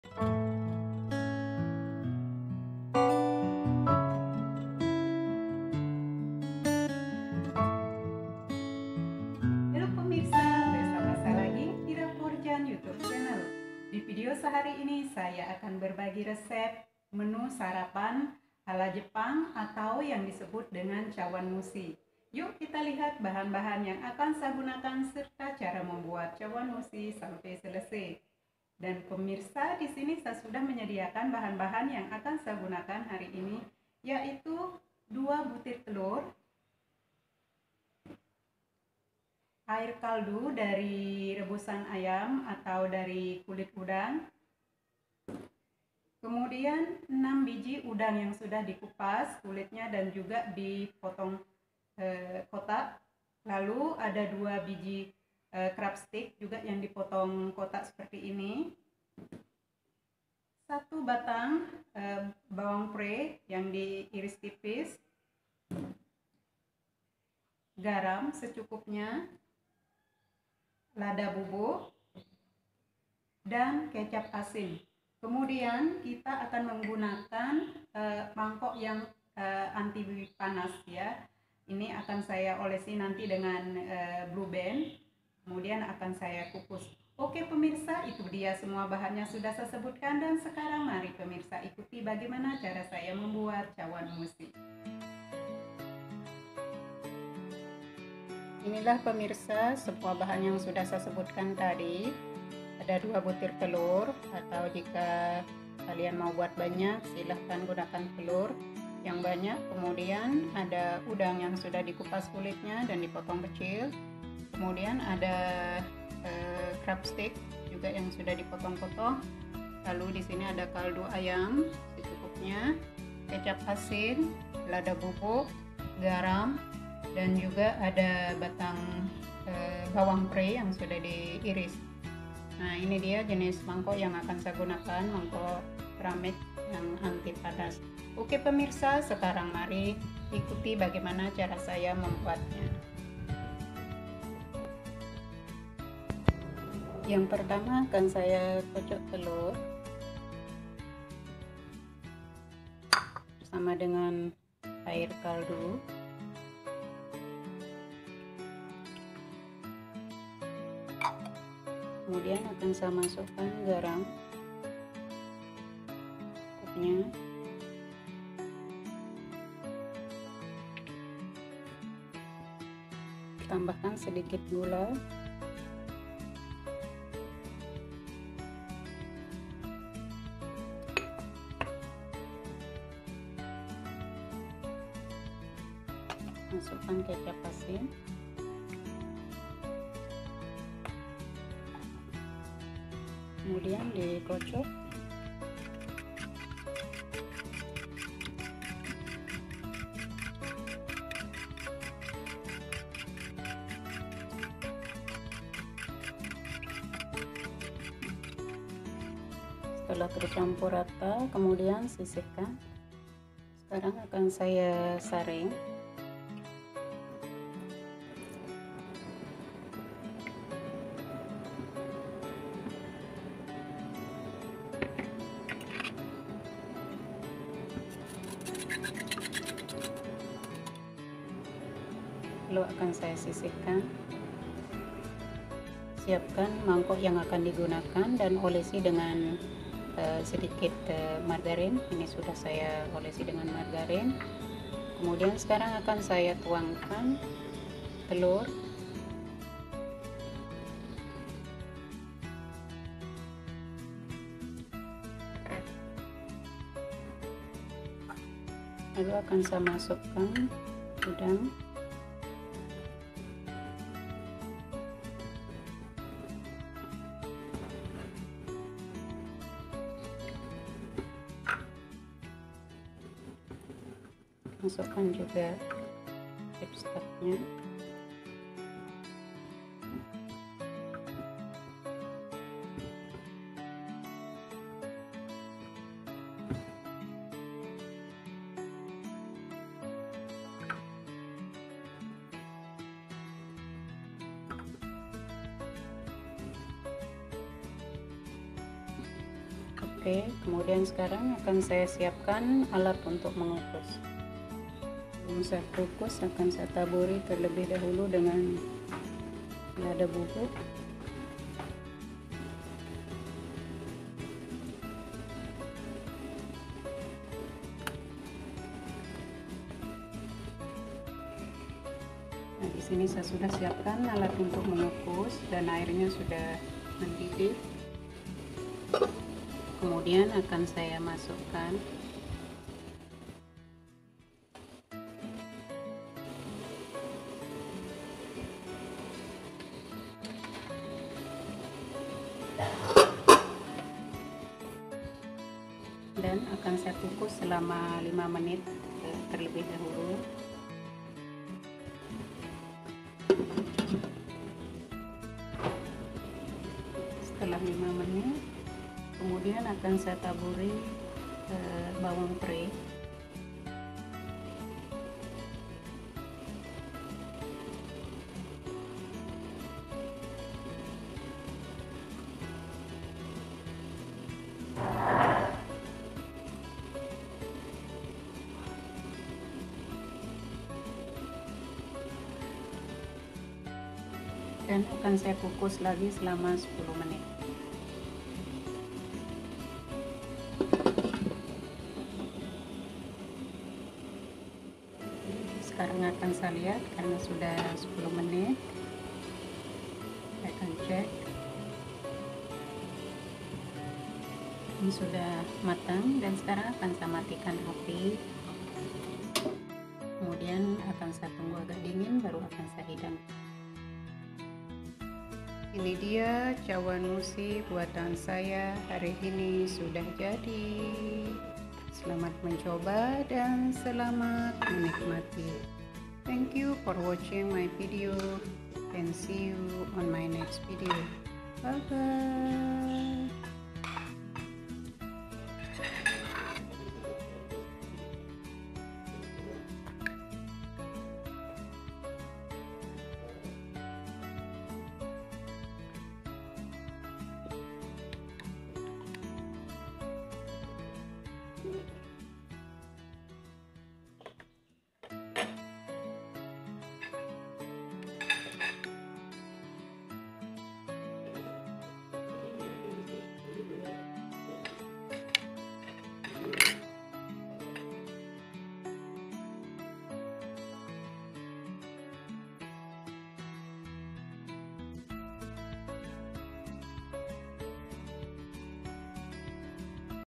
Resep CHAWANMUSHI Khas Jepang, Telur Kukus yang Praktis untuk Sarapan Sehat dan Bergizi
Hello Pemirsa hari ini saya akan berbagi menu sarapan sehat yang praktis buat keluarga tercinta di rumah yaitu Chawanmushi, menu TelurKukus ini rasanya sangat enak dan mudah dibuat.
CHAWANMUSHI adalah salah satu hidangan menggugah selera yang berasal dari Jepang dan sangat cocok sekali untuk menu sarapan karena kaya gizi dan sehat.
Bahan yang di gunakan sangat mudah seperti: Telur, Air Kaldu, Udang, Crab Stick, Batang Prei atau Jamur, Lada Bubuk, Gula, Garam dan Kecap Asin. Kemudian peralatan masak yang digunakan seperti: Mangkok Keramik anti panas dan alat untuk mengukus.
Untuk lebih jelas tentang cara membuat Chawanmushi atau TelurKukus, silahkan kalian ikuti resep dari IraHomeCooking dan ditonton videonya selesai.
Terimaksih dan Selamat Mencoba
xxx

Timestamp:
0:00   Intro
01:35 Bahan-Bahan (Ingredients)
02:25 Mengulang Semua Bahan (Repeat All Ingredients)
04:54 Mencampur Telur Dengan Air Kaldu Ayam (Mix Eggs And Chicken Stock)
05:21 Tuangkan Kedalam Mangkok Tahan Panas (Pour Into a Heatproof Bowl)
06:54 Mulai Mengukus (Start Steaming)
07:46 Chawanmushi Siap Dihidangkan (Ready To Served)